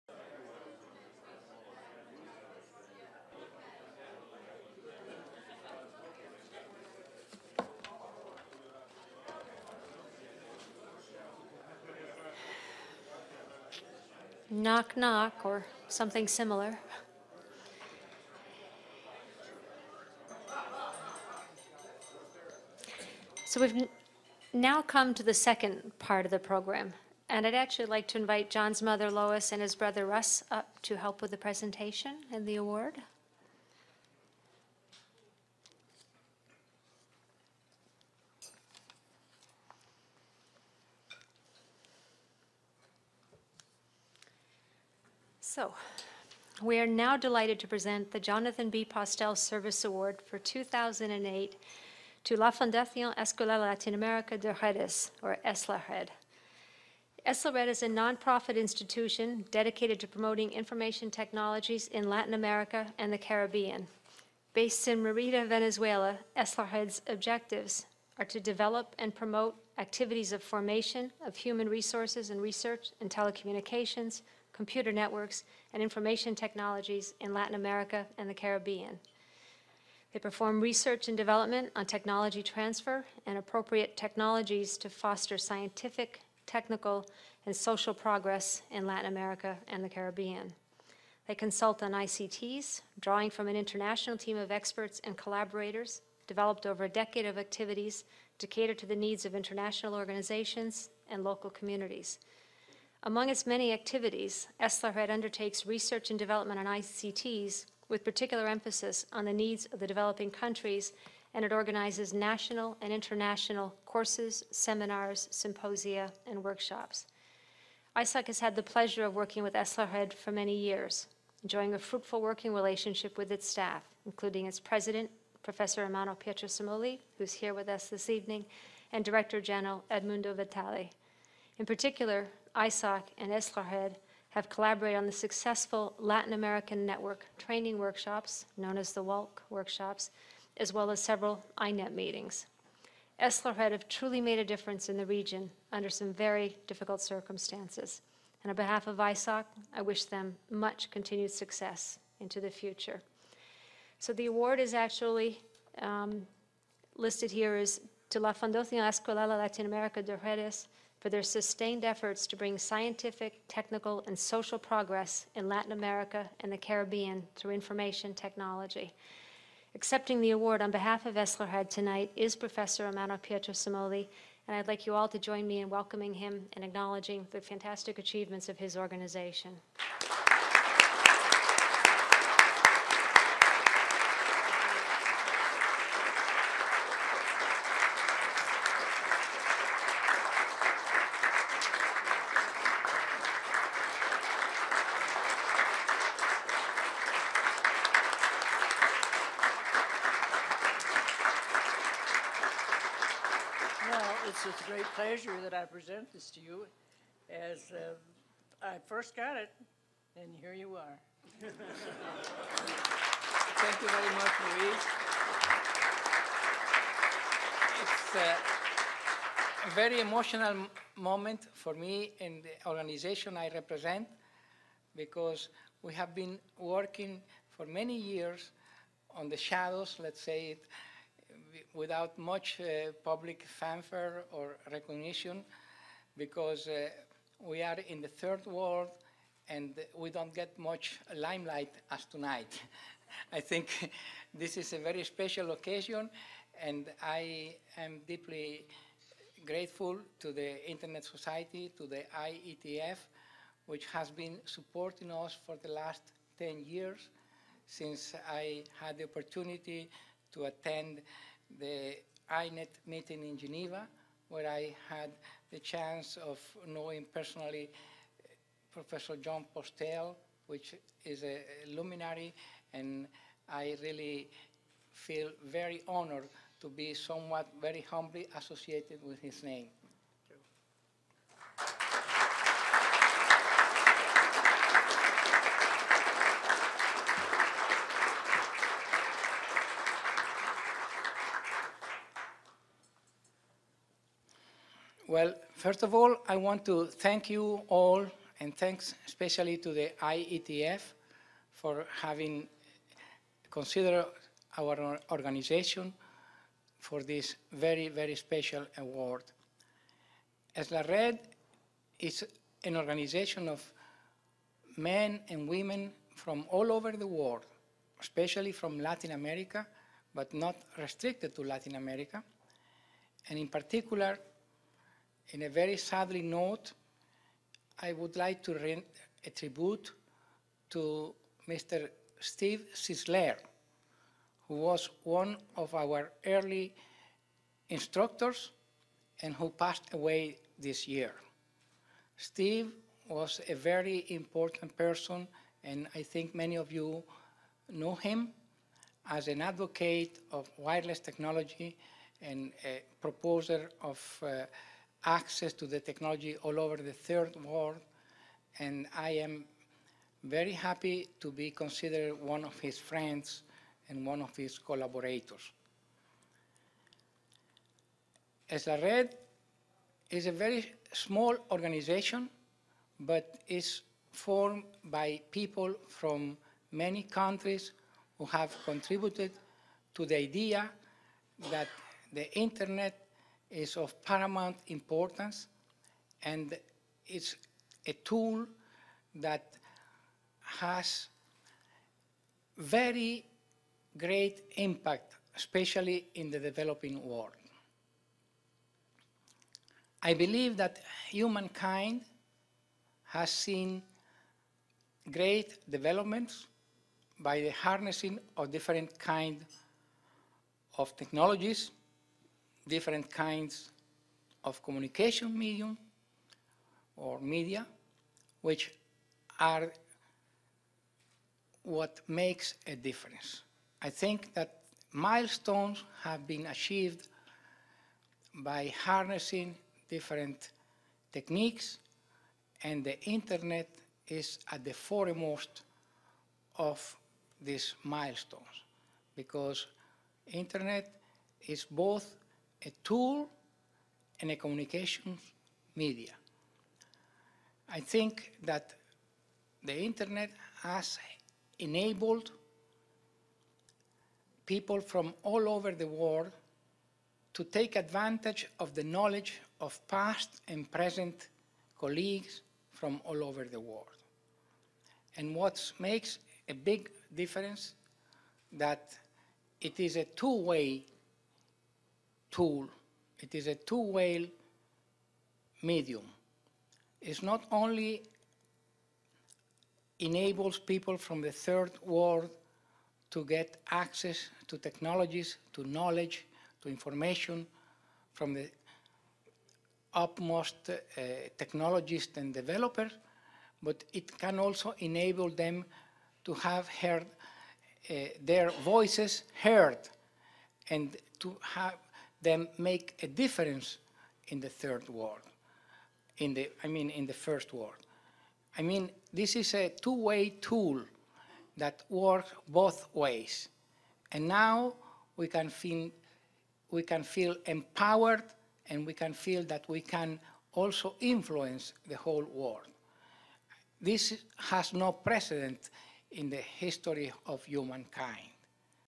Part 1:Coveted Jonathan B. Postel Service Award Granted to EsLaRed
Closed captioned/subtitled in English.

For the first time in its 10-year history, the Jonathan B. Postel Service Award has been granted to an organisation rather than to an individual. And while many organisations have successfully brought the Internet to life in rural, remote, or developing regions, few capture the spirit of Jon Postels legacy quite like La Fundación Escuela Latinoamericana de Redes.